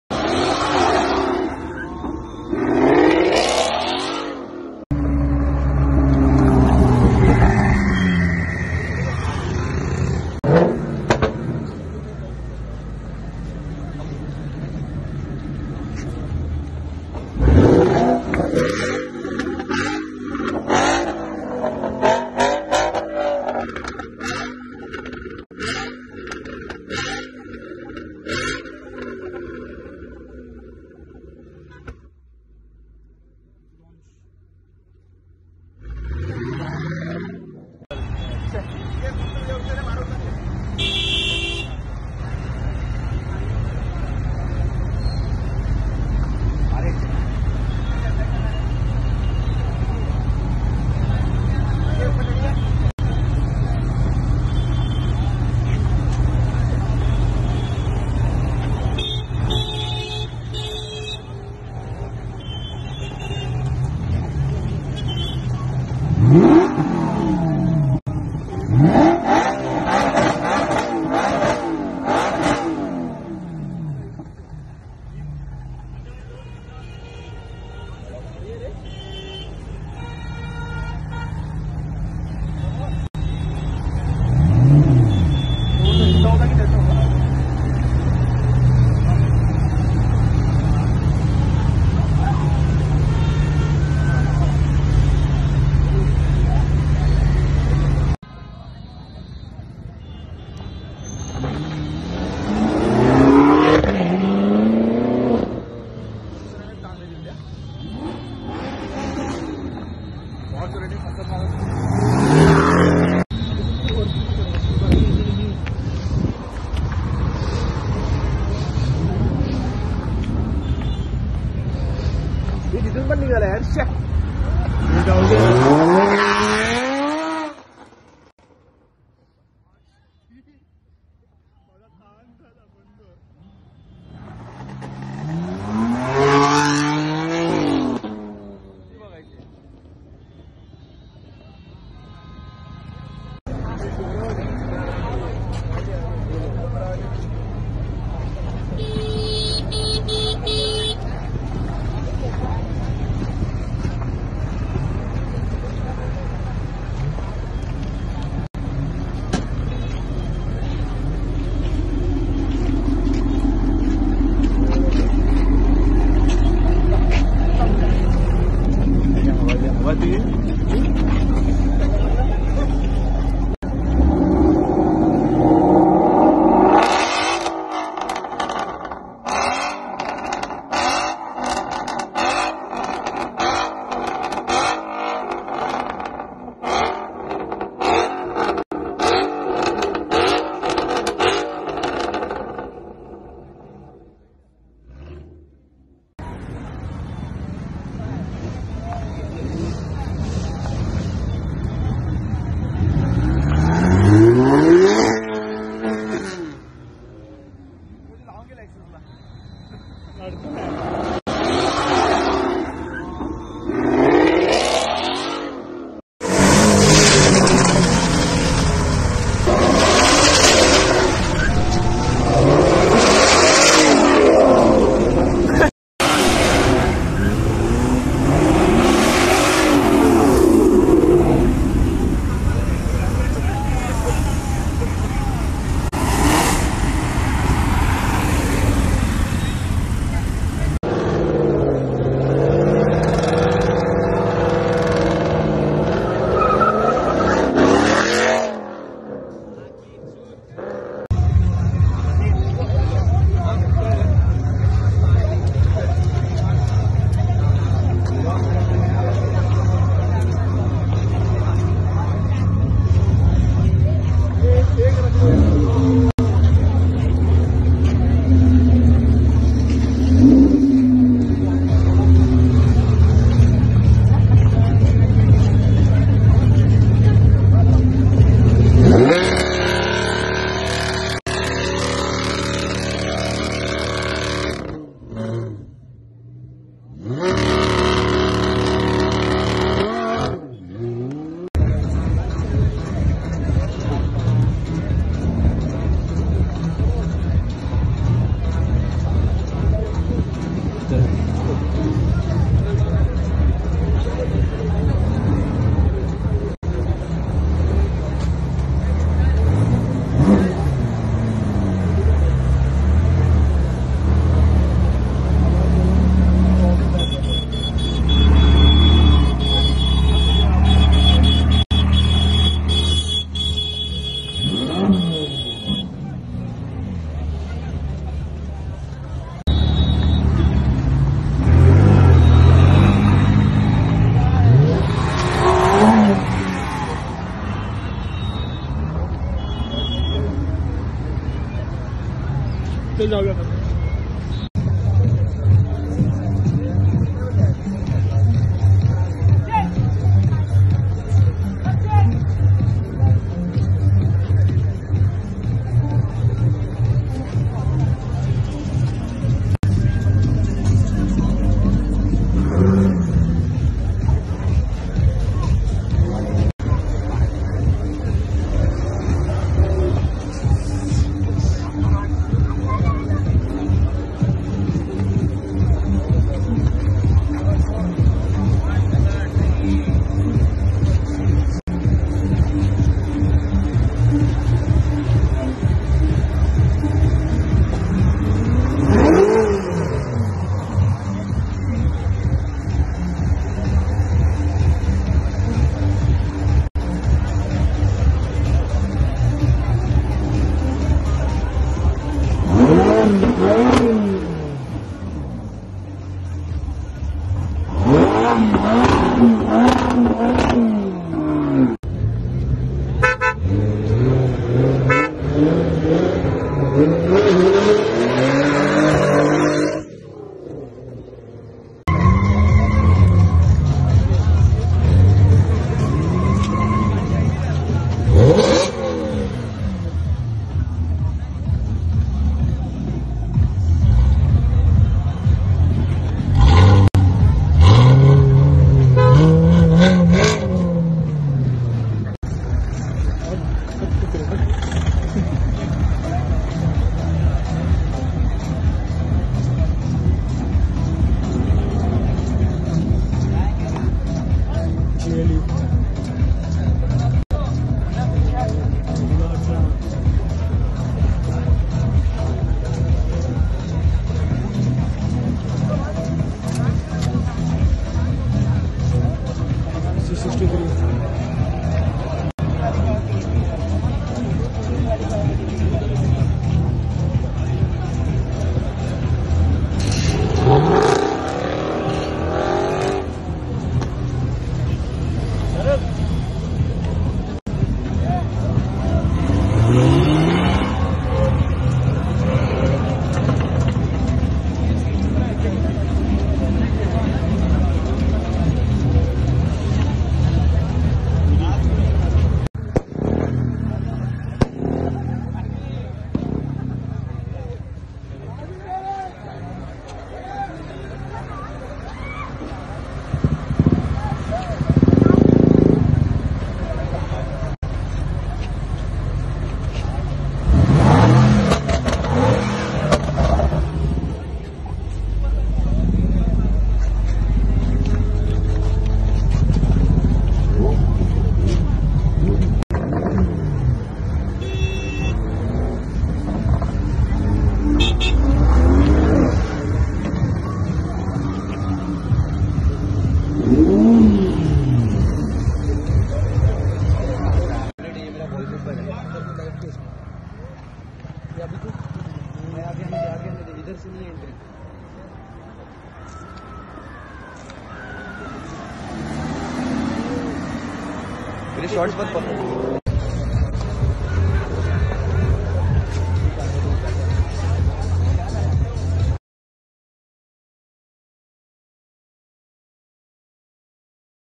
Loud Supercars in Mumbai || Republic Day 2020 || Public Gone Crazy for SuperCars
Republic Day supercars show, drive in mumbai.
26/01/2020.